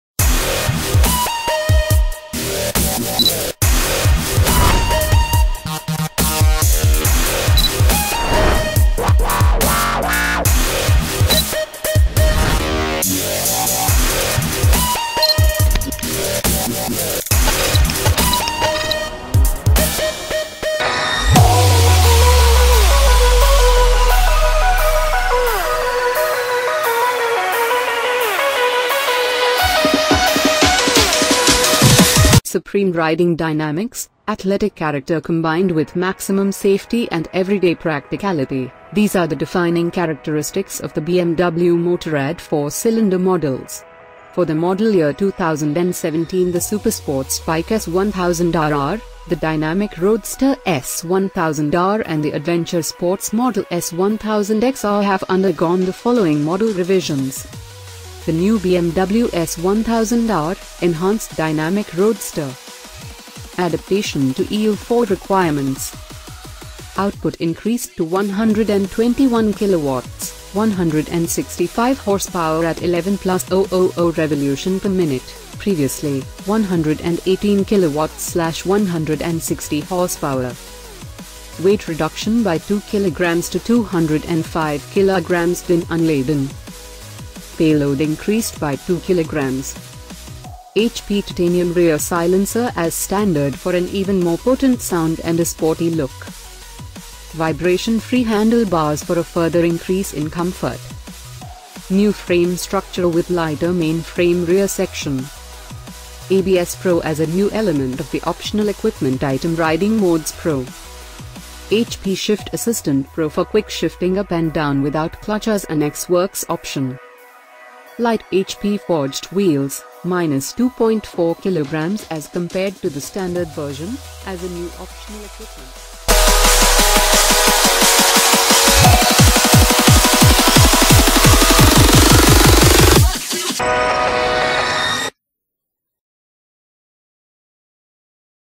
Hot News Low Price 2017 BMW S1000R
Hot News Low Price 2017 BMW S1000R - Supreme riding dynamics, athletic character combined with maximum safety and everyday practicality – these are the defining characteristics of the BMW Motorrad 4-cylinder models. For the model year 2017 the supersports bike S 1000 RR, the dynamic roadster S 1000 R and the adventure sports model S 1000 XR have undergone the following model revisions:

 $13,495